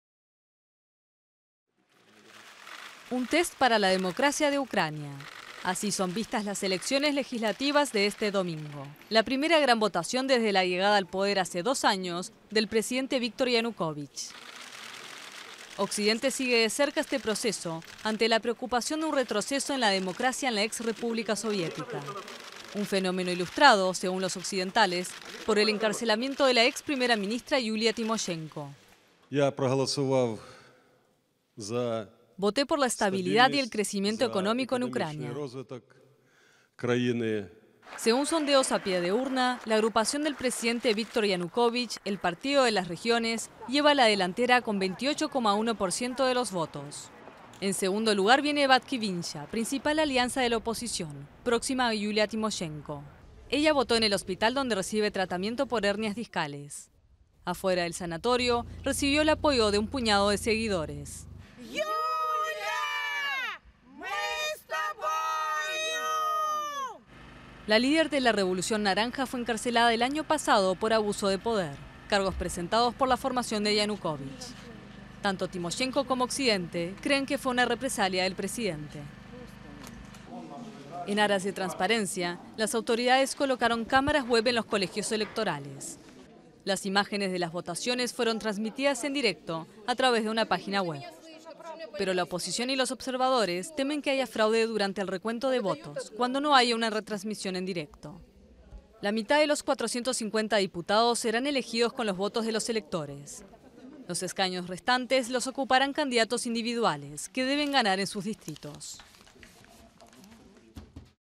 Test de la democracia en Ucrania
El partido del presidente Viktor Yanukovich encabeza las legislativas del domingo en Ucrania, vistas como una prueba para la democracia en el país, principalmente tras el encarcelamiento de la exprimera ministra Yulia Timoshenko.